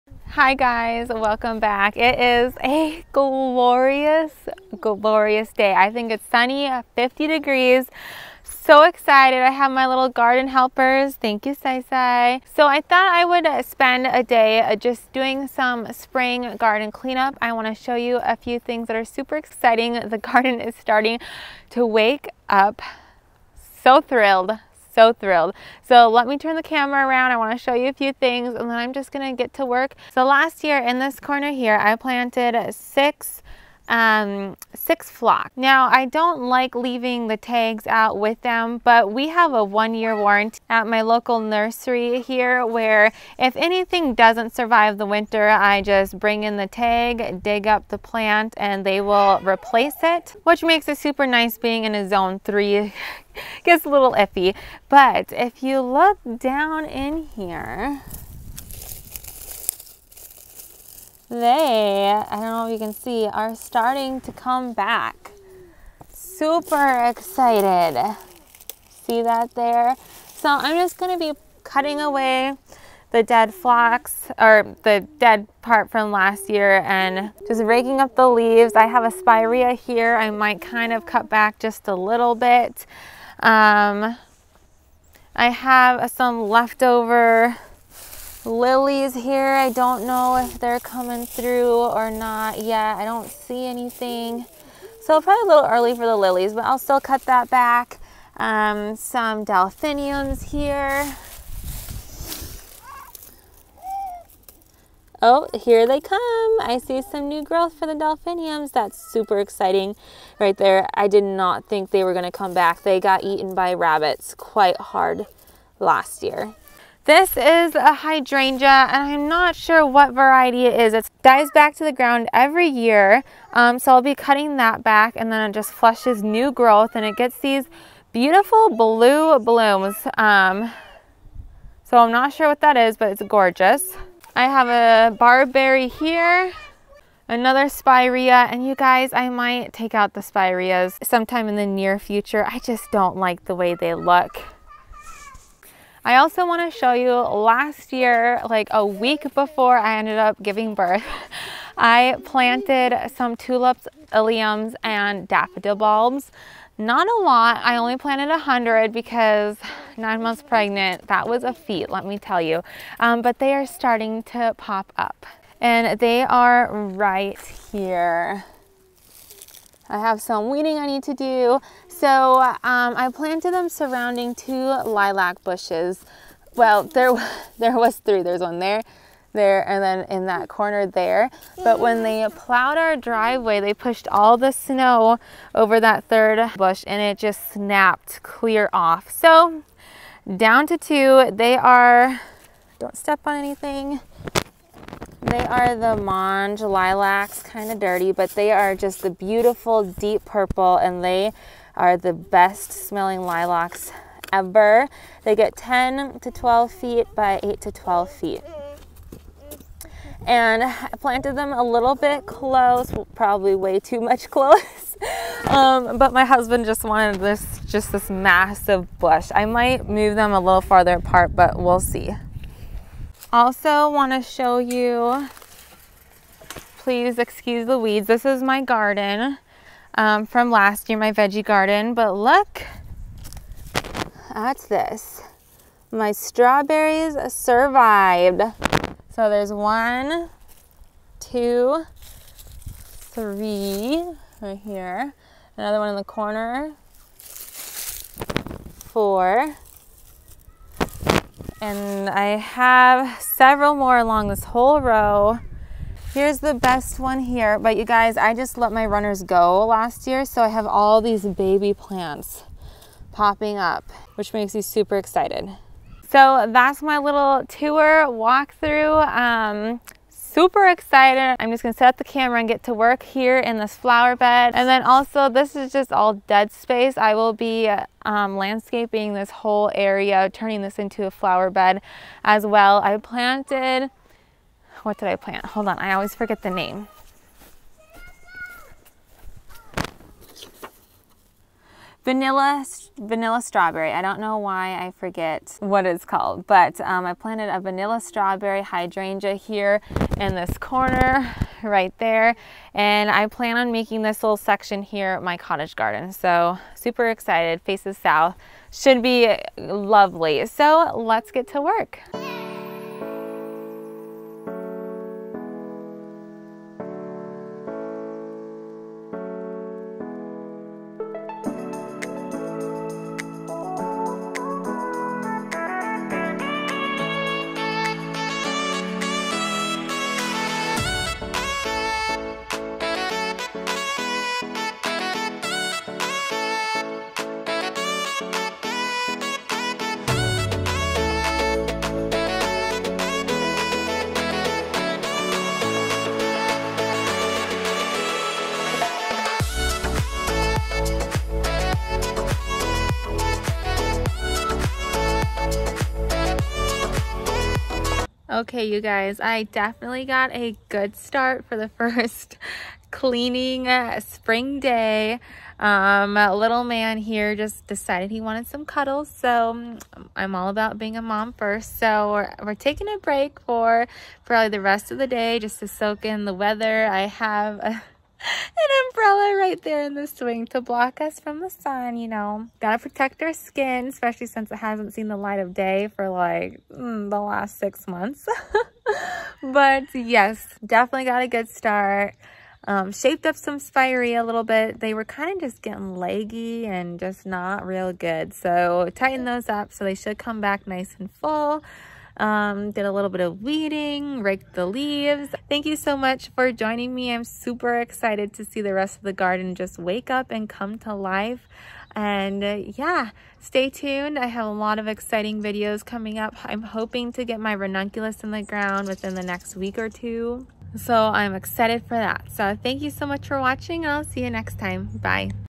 Spring Is Here! Spring Garden Clean Up! | Simply Bloom
Spring is FINALLY here!  I didn't think it was every going to show up and part of me is still not fully believing that spring is hear just incase winter decides to make an appearance again!  But while I have the nice weather it is time to get some spring cleaning done around the garden!